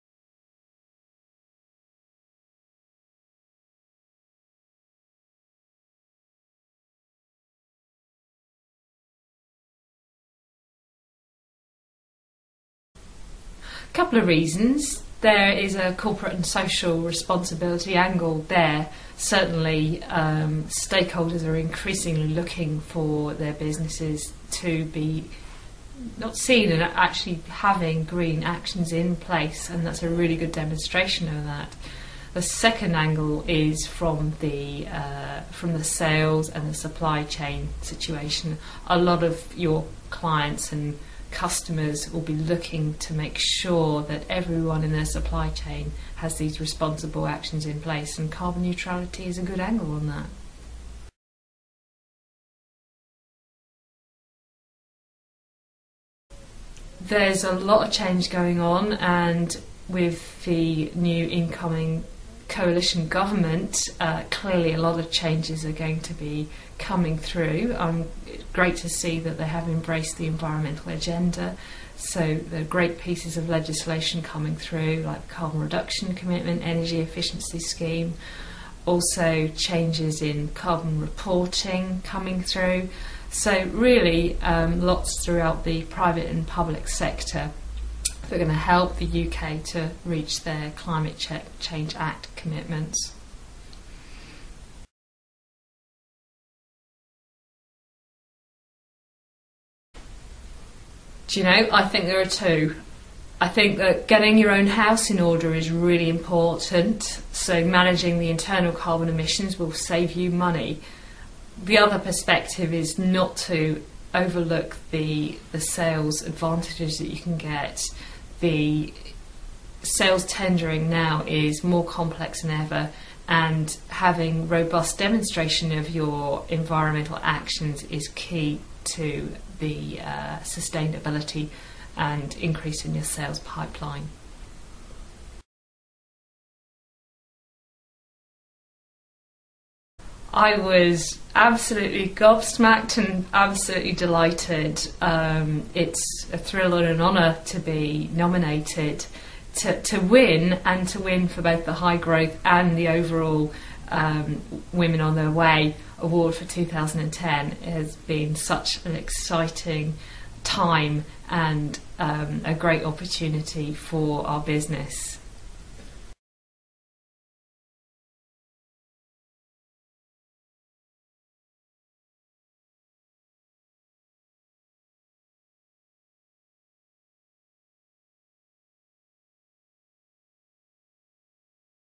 Dr Wendy Buckley - Carbon Footprint Ltd
Interview with Dr Wendy Buckley, co founder and director of Carbon Footprint Ltd, on carbon management